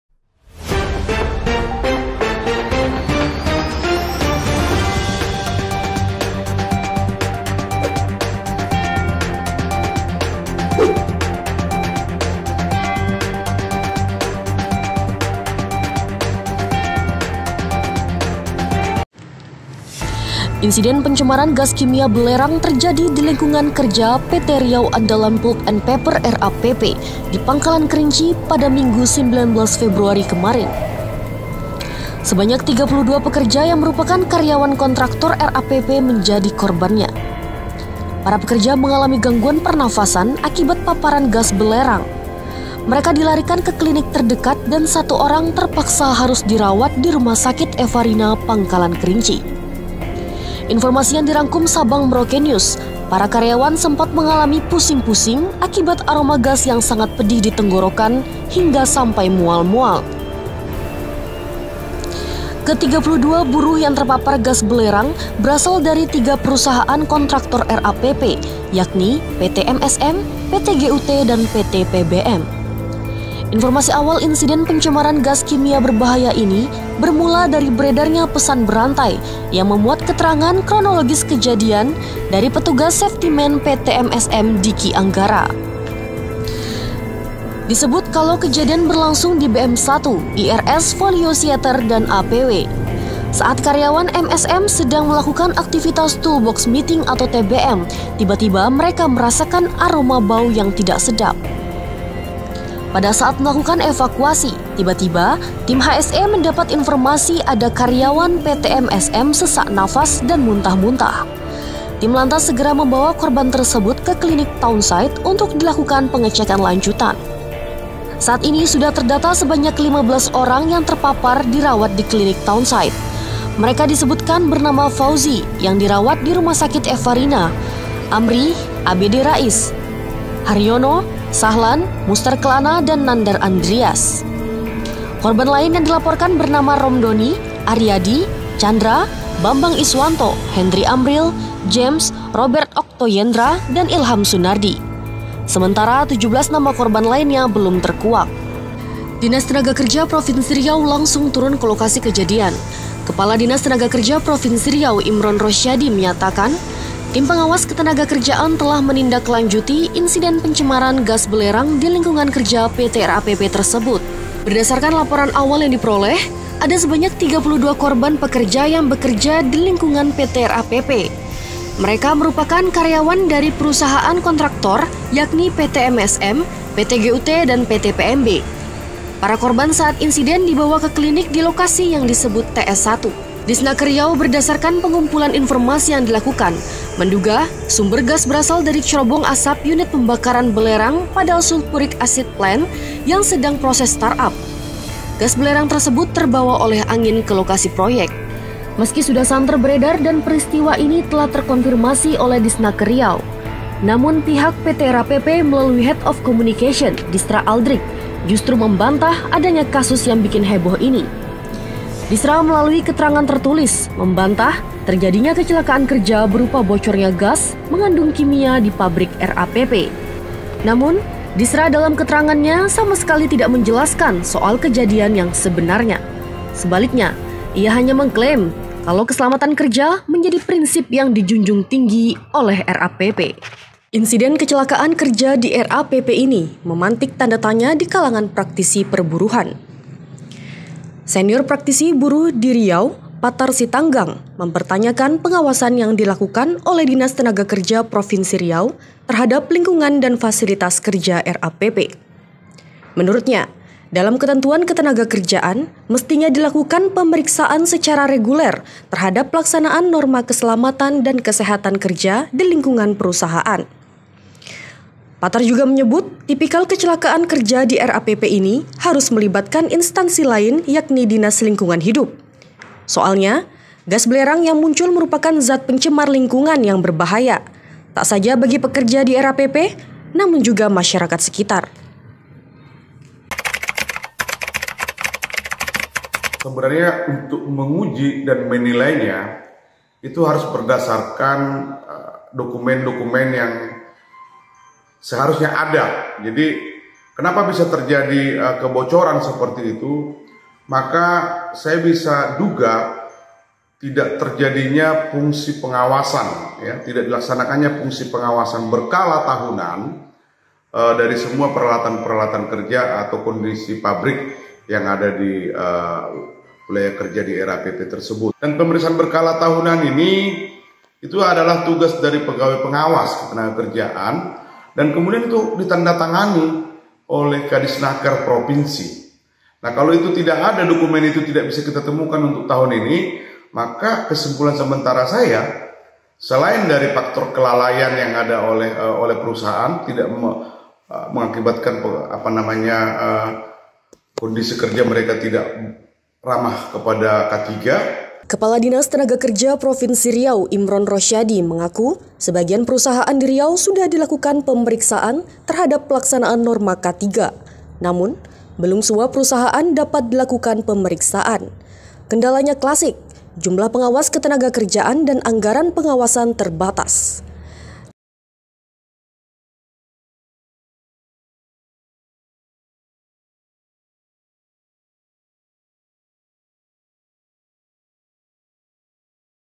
Pencemaran Gas Belerang di Lingkungan Kerja, Pejabat Humas PT RAPP Membantah: Apa yang Terjadi?
Insiden pencemaran gas kimia belerang terjadi di lingkungan kerja PT Riau Andalan Pulp and Paper RAPP di Pangkalan Kerinci pada Minggu 19 Februari kemarin. Sebanyak 32 pekerja yang merupakan karyawan kontraktor RAPP menjadi korbannya.

Para pekerja mengalami gangguan pernafasan akibat paparan gas belerang. Mereka dilarikan ke klinik terdekat dan satu orang terpaksa harus dirawat di Rumah Sakit Efarina, Pangkalan Kerinci.

Informasi yang dirangkum SabangMerauke News, para karyawan sempat mengalami pusing-pusing akibat aroma gas yang sangat pedih di tenggorokan hingga sampai mual-mual.  Ke 32 buruh yang terpapar gas belerang berasal dari 3 perusahaan kontraktor RAPP yakni, PT MSM, PT GUT dan PT PBM.

Informasi awal insiden pencemaran gas kimia berbahaya ini bermula dari beredarnya pesan berantai yang memuat keterangan kronologis kejadian dari petugas Safety Man PT MSM, Dicky Anggara.

Disebut kalau kejadian berlangsung di BM1, IRS, Folio Shieter dan APW. Saat karyawan MSM sedang melakukan aktifitas Tool Box Meeting (TBM), tiba tiba mereka merasakan aroma bau yang tidak sedap.

Pada saat melakukan evakuasi, tiba-tiba tim HSE mendapat informasi ada karyawan PT MSM sesak nafas dan muntah muntah. Tim lantas segera membawa korban tersebut ke klinik townsite untuk dilakukan pengecekan lanjutan.

Dinas Tenaga Kerja Provinsi Riau langsung turun ke lokasi kejadian. Kepala Dinas Tenaga Kerja Provinsi Riau, Imron Rosyadi menyatakan, tim pengawas ketenagakerjaan telah menindaklanjuti insiden pencemaran gas belerang di lingkungan kerja PT RAPP tersebut.

Disnaker Riau berdasarkan pengumpukan informasi yang dilakukan, menduga sumber gas berasal dari cerobong asap unit pembakaran belerang pada Sulphuric Acid Plant yang sedang proses start-up. Gas belerang tersebut terbawa oleh angin ke lokasi proyek.

Meski sudah santer beredar dan peristiwa ini telah terkonfirmasi oleh Disnaker Riau, namun Pihak PT RAPP melalui Head Of Communications, Disra Alldrick justru membantah adanya kasus ini.

Disra melalui keterangan tertulis, membantah terjadinya kecelakaan kerja berupa bocornya gas mengandung kimia di pabrik RAPP.

Namun Disra dalam keterangannya sama sekali tidak menjelaskan soal kejadian yang sebenarnya. Sebaliknya, ia hanya mengklaim kalau keselamatan kerja menjadi prinsip yang dijunjung tinggi oleh RAPP. (*)